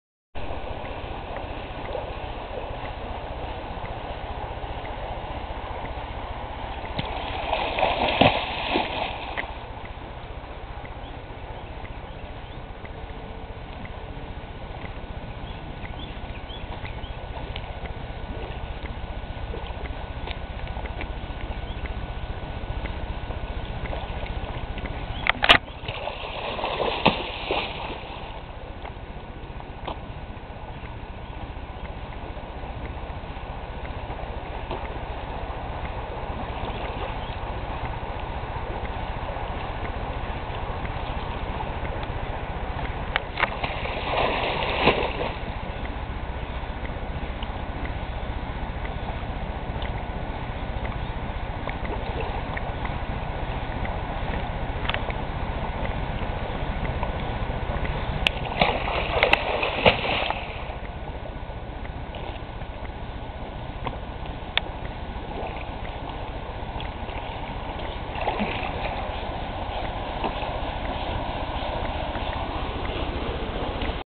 Merman Jesse @ Juniper Springs florida
FINISHED!   Swimming in the tail that I MADE FOR MYSELF.

This was filmed at beautiful Juniper Springs in the Ocala Nat. Forest in Florida.